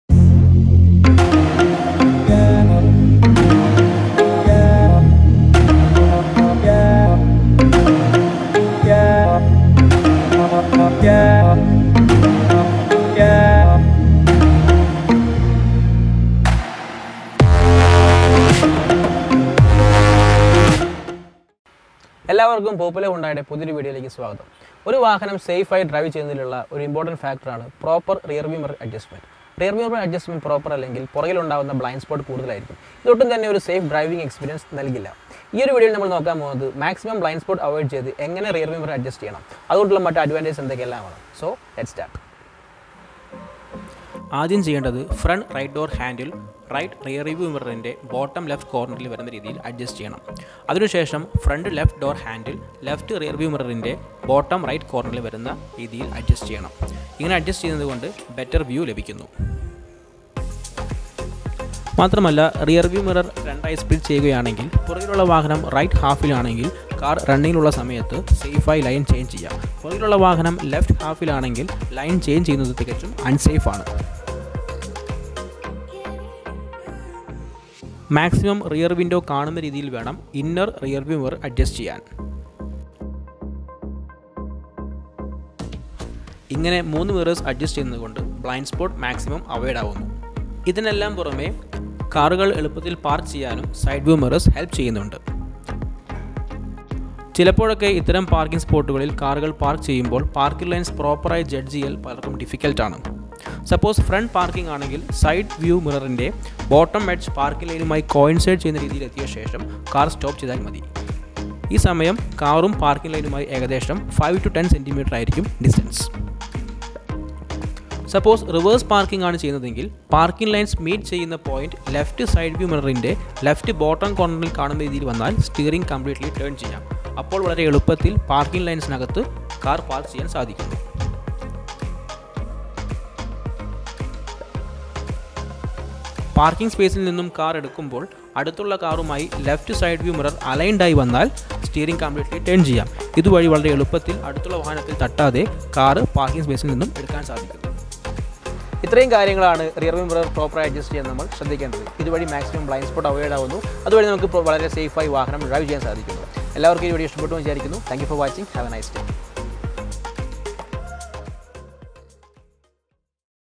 PROPER REAR VIEW MIRROR ADJUSTMENT | SAFETY TIPS | SAFE DRIVING | POPULAR HYUNDAI
This video deals with explaining the importance of proper rear view mirror adjustments which enables safe driving experience by avoiding maximum blind spot. Also, this video shows easy and simple tips for proper parking of your vehicle.